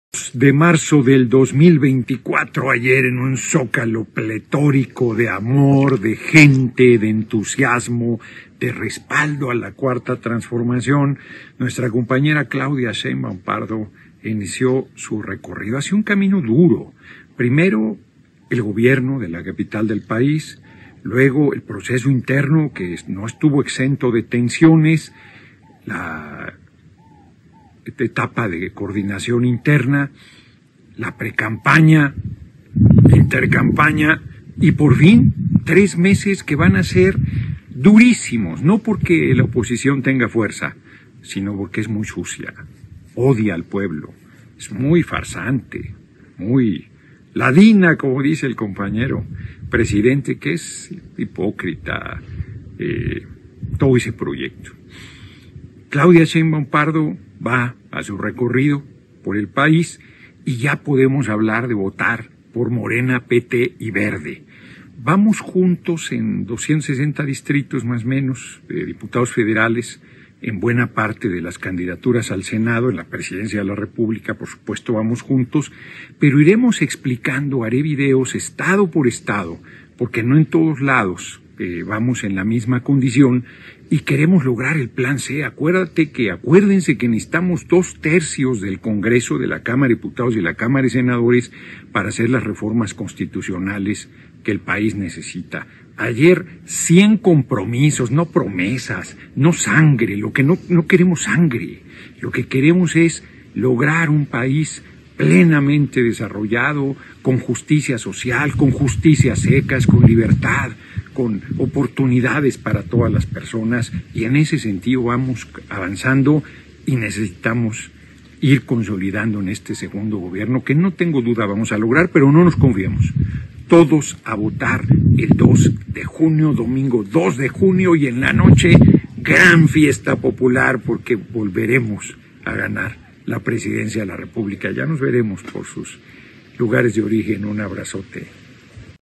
Noroña La oposicion es LADINA TRAMPOSA Y CORRUPTA
El Diputado Noroña invita a los seguidores de la cuarta transformacion a no confiarnos y salir a votar el 2 de Junio del 2024 para lograr llegar al PLAN C